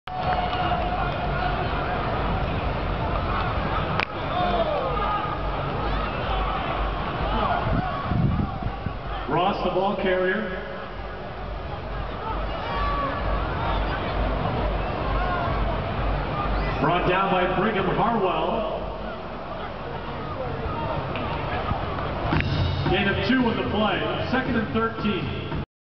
United Football League California Redwoods vs. New York Sentinels @ AT&T Park San Francisco 2009
California Redwoods vs. New York Sentinels of the United Football League at AT&T Park in San Francisco.